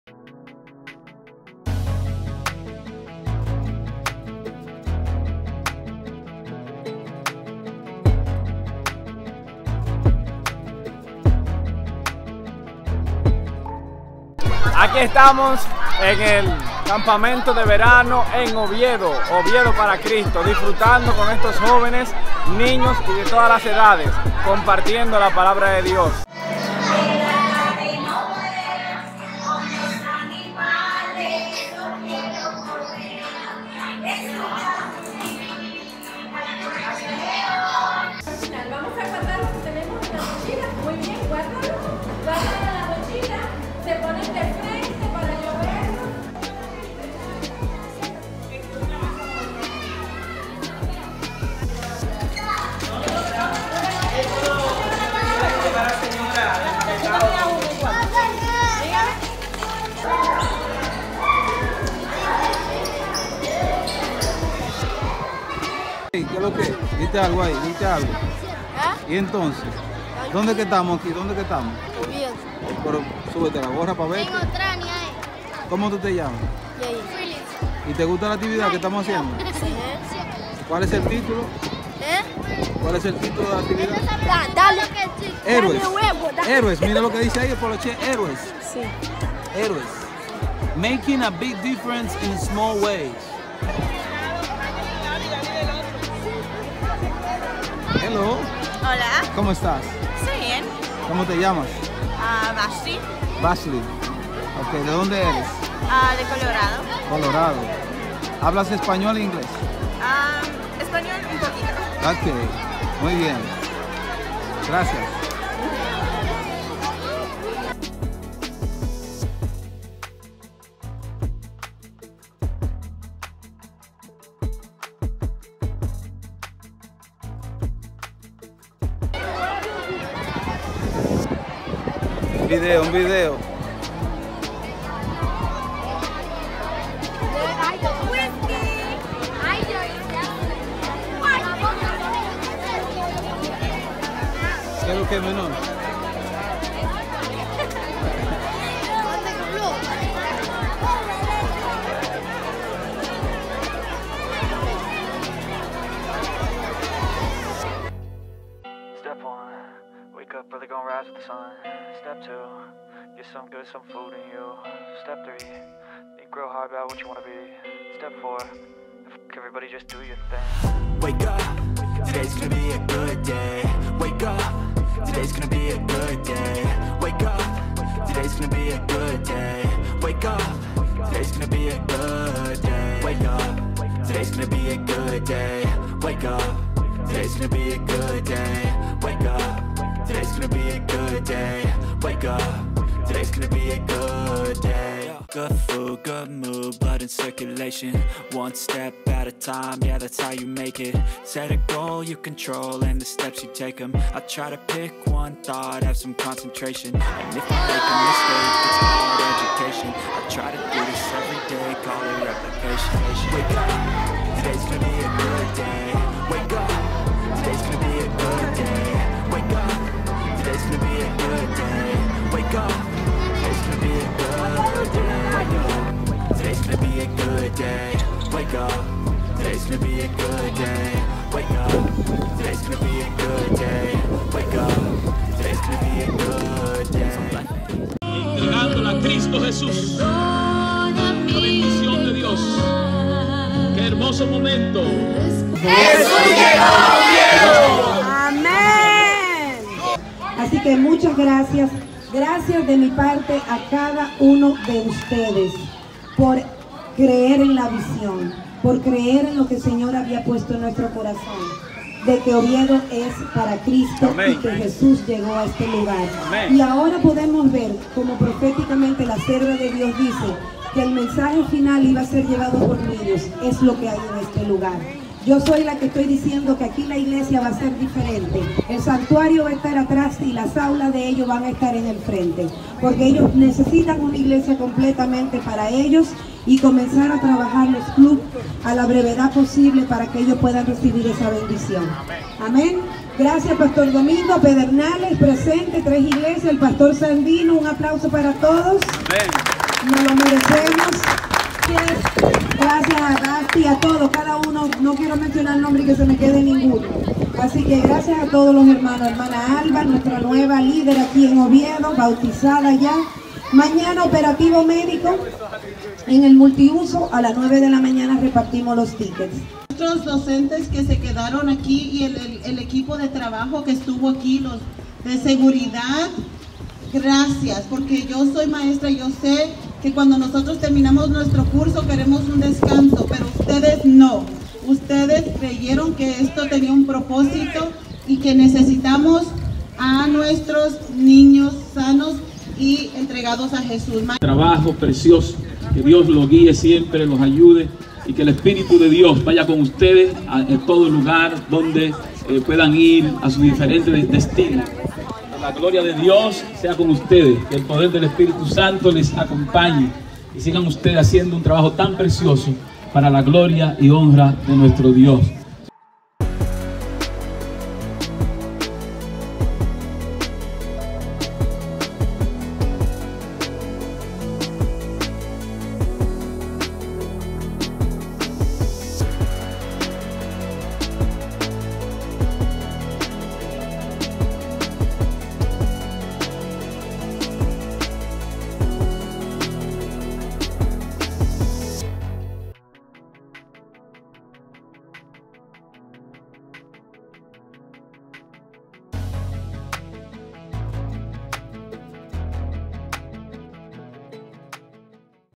Oviedo Para Cristo 2022
Campamento de Verano para niños en la comunidad de Oviedo.
Oviedo es un municipio situado en la provincia de Pedernales, República Dominicana.

Héroes "Making a Big Difference in Small Ways"
Esto fue un viaje misionero de Gospel Travel Mission.

Video Producido y Editado por:
Francisco Martinez

Equipo usado para la realizacion de este video: 🎥🎬
Iphone 8 normal 📱
Mini Drone (DJI Mini SE) 🚁